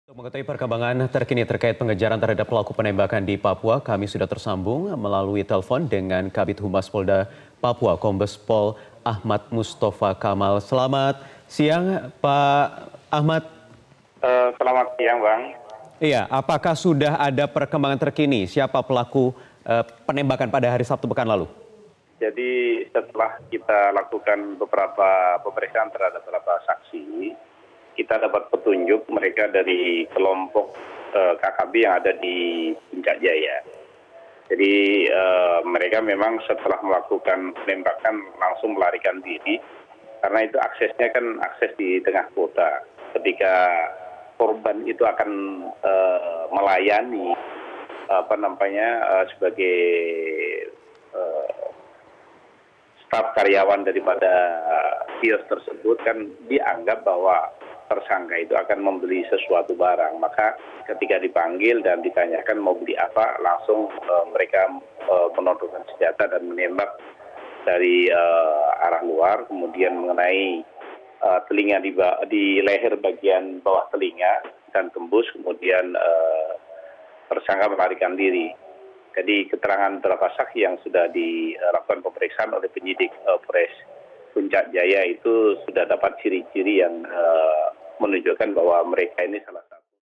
Polisi Kantongi Nama Penembak di Puncak Jaya
Kabid Humas Polda Papua, Kombes Pol A. M. Kamal mengatakan pelaku penembakan penjaga kios di Puncak Jaya, Papua berasal dari kelompok kriminal bersenjata (KKB). Mereka diketahui sudah melakukan persiapan untuk melancarkan aksi ini. Mereka sengaja melakukannya untuk menciptakan situasi tidak kondusif.